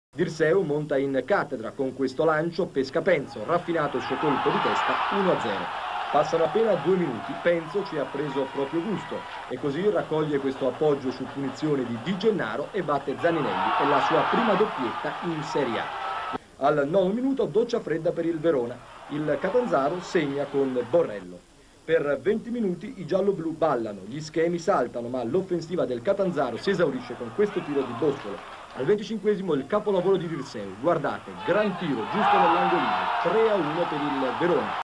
1982/83, Serie A, Verona - Catanzaro 3-1 (07)
Nel Campionato di Serie A del 1982/83 nella settima giornata del girone di Andata, Verona - Catanzaro finisce 3-1.